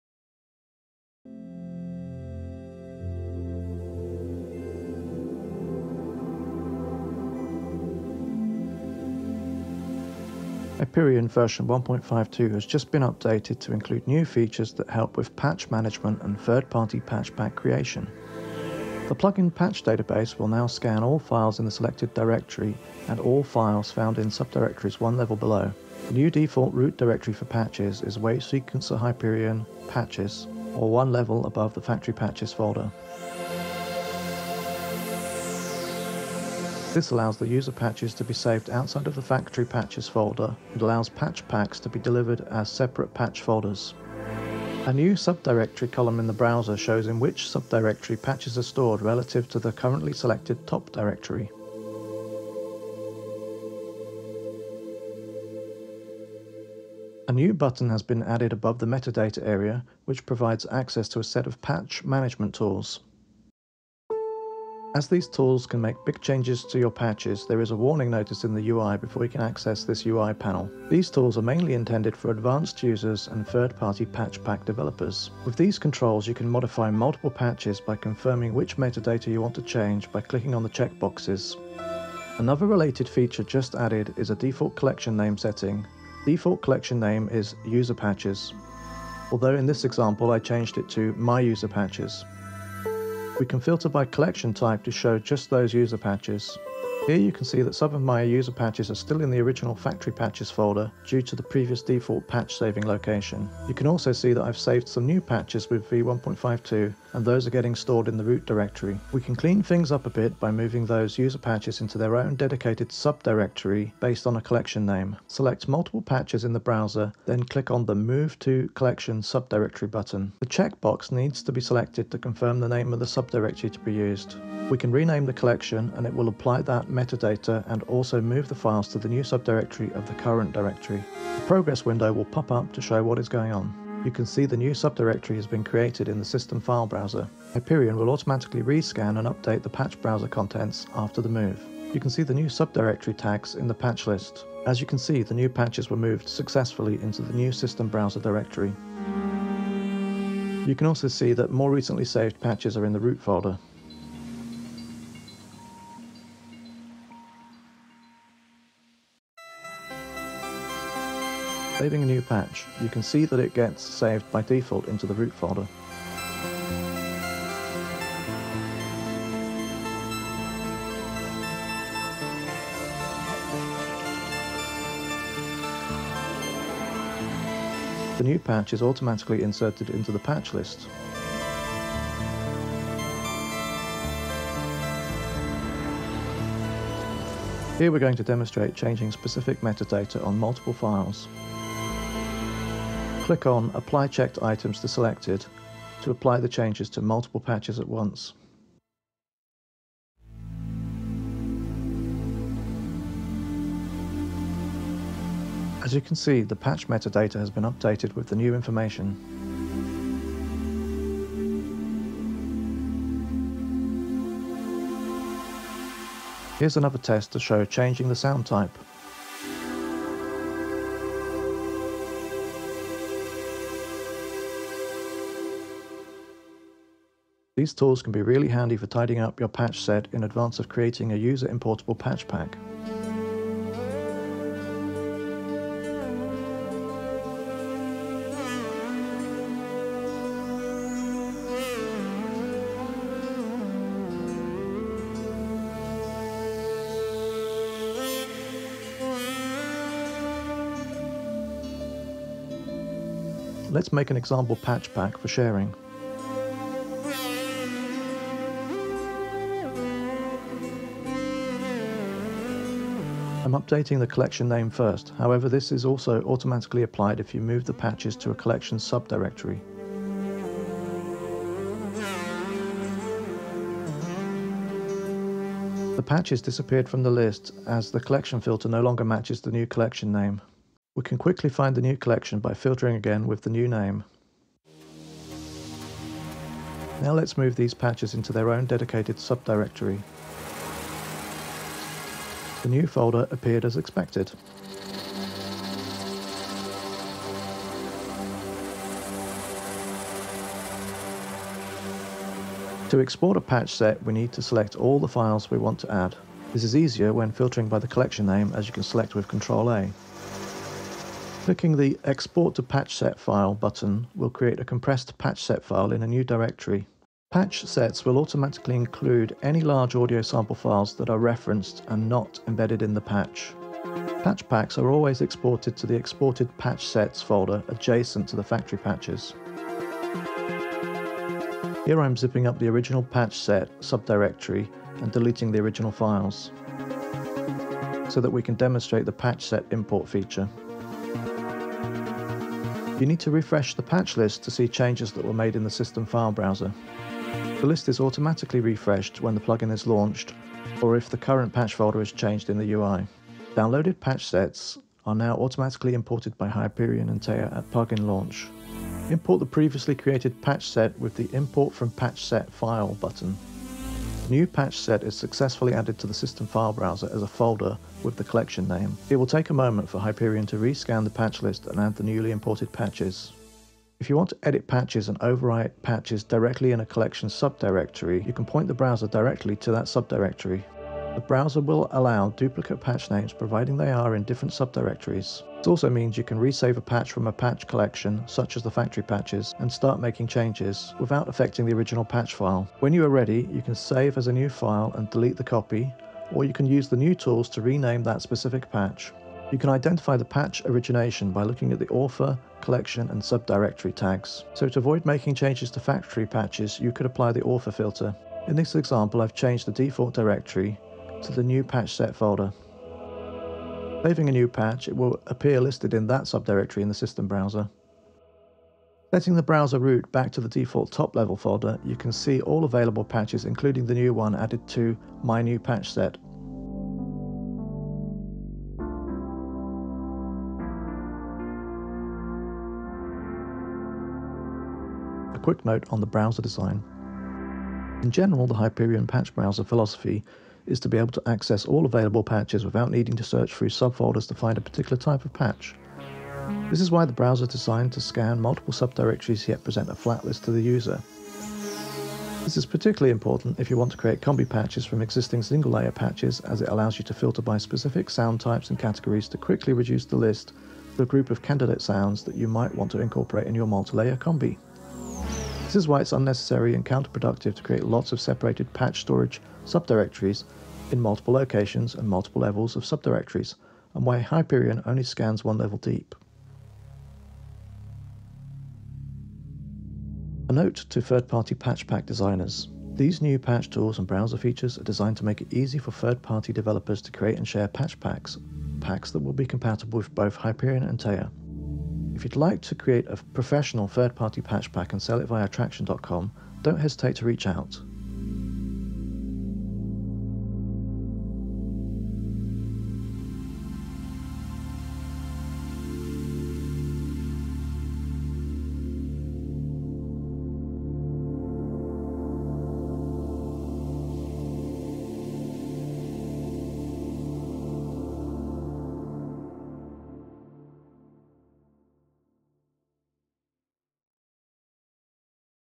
New patch management features - Hyperion Synth v1.52
New tools to manage multiple patches at the same time such as modifying specific meta-data tags, moving patches into collection sub-directories, and exporting or importing compressed patch sets.

These are 'power users' tools - so if you are not sure what you are doing, it's advisable to back up your patches first!

The latest version of Theia (v1.09 also supports the new sub-directory scanning features and auto-import of patch set files, and will automatically update the browser root directory from Patches/FactoryPatches to Patches/).